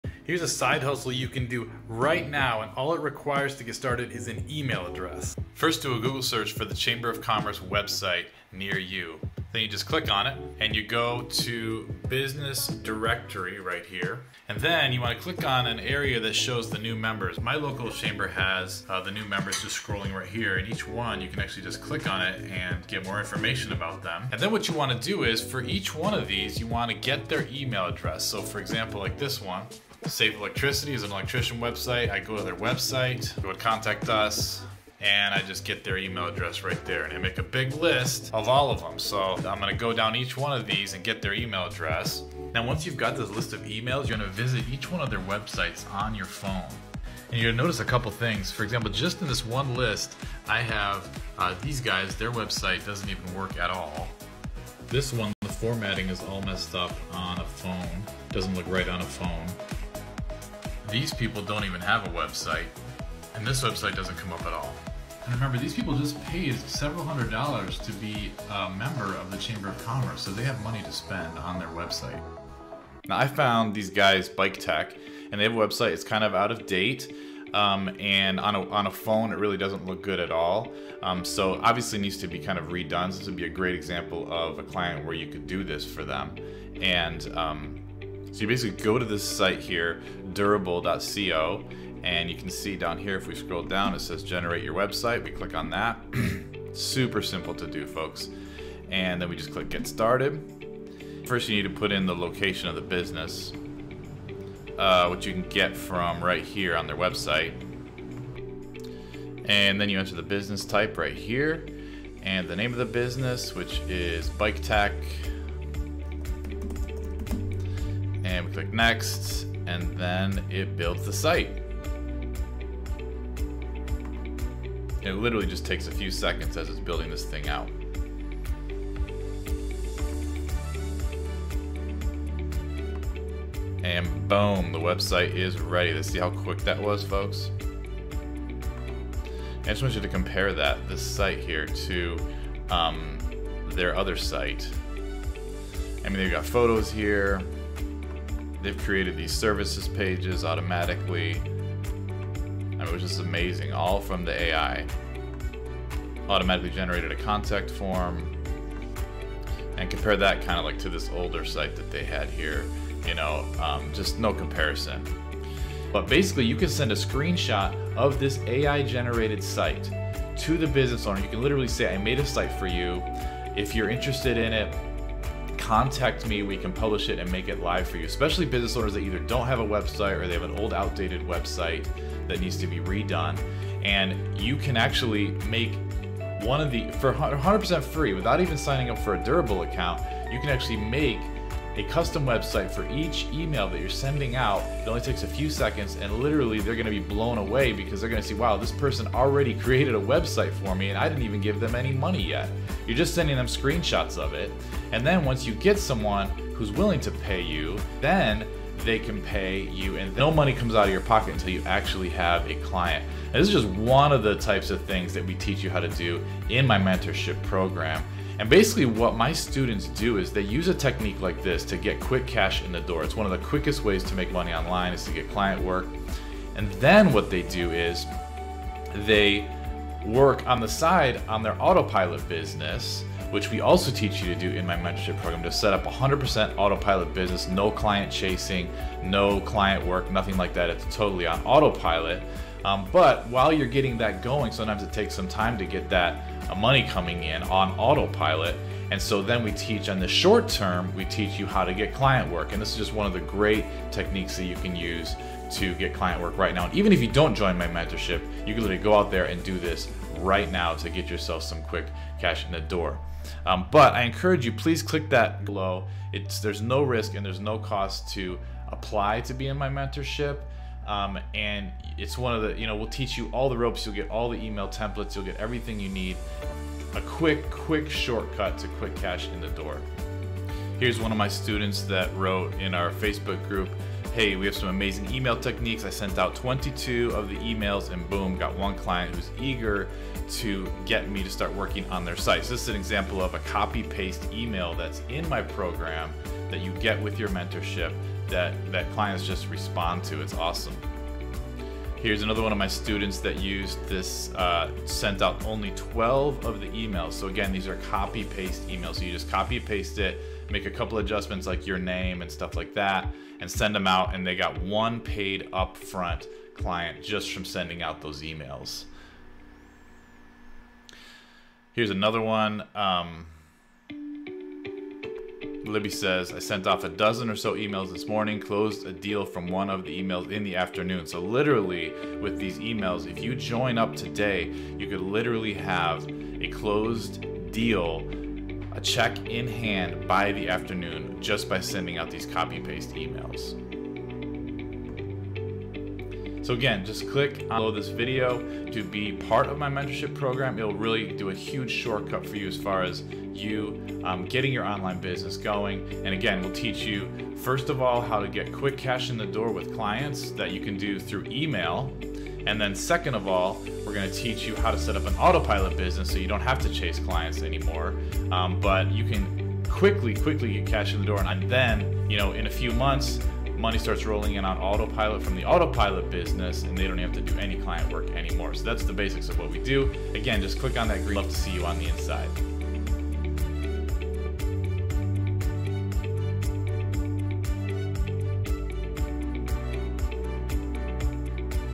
Little Known Way to Make $$ With Local Chamber of Commerce Website
Make Money Online with this free technique. Some have used this tactic to start an online business and make a full time income online!